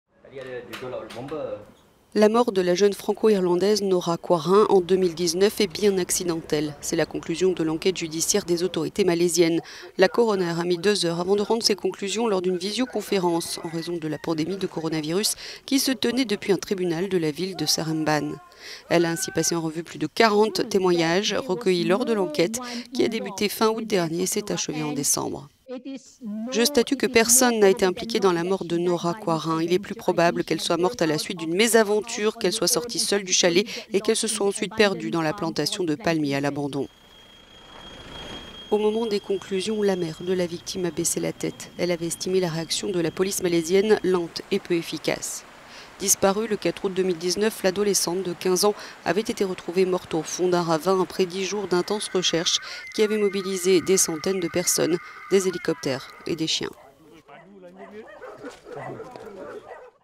Malaisie : la mort de Nora Quoirin est bien accidentelle
Selon les conclusions de l'enquête, la mort de la jeune Franco-Irlandaise, Nora Quoirin, dont le corps avait été retrouvé dans la forêt en Malaisie dix jours après sa disparition en 2019, est due à "une mésaventure" dans la jungle et non à un meurtre ou à une agression sexuelle.